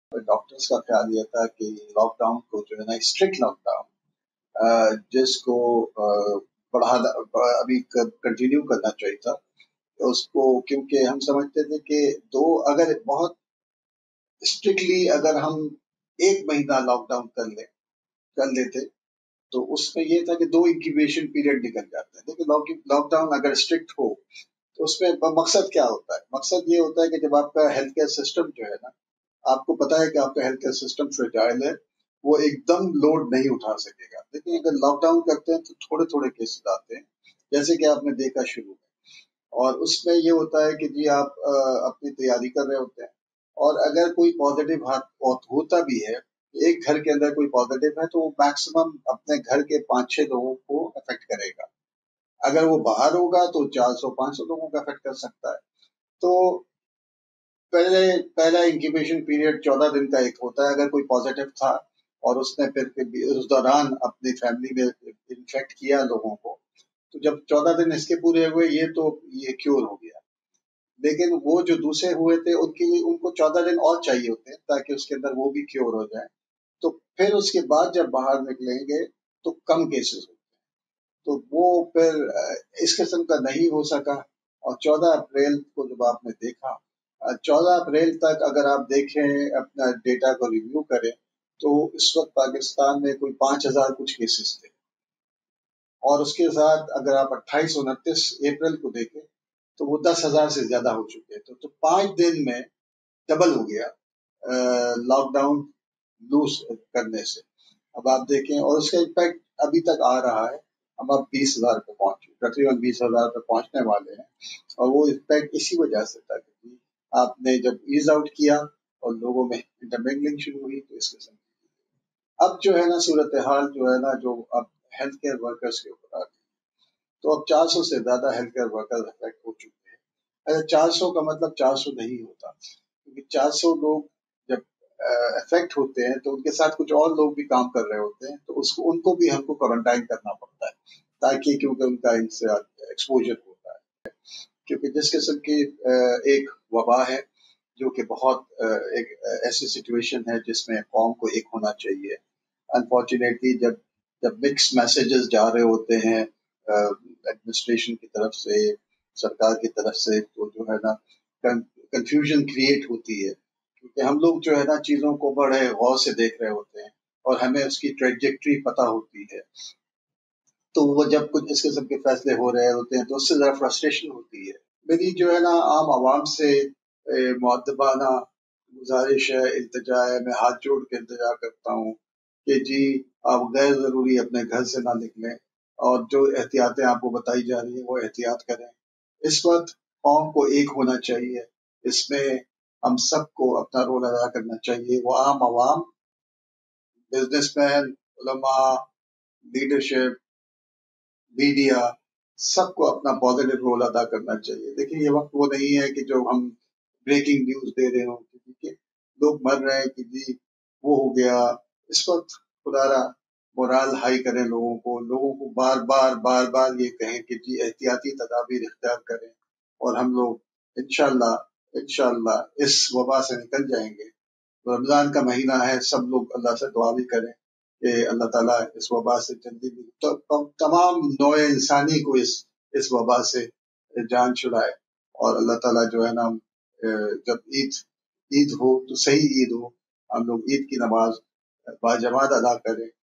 Why are Doctor asking for a lockdown? Dr Bari, CEO of Indus Hospital, explains - TPE Clips
Dr Bari, the CEO of Indus Hospital, explains why doctors are asking for a lockdown.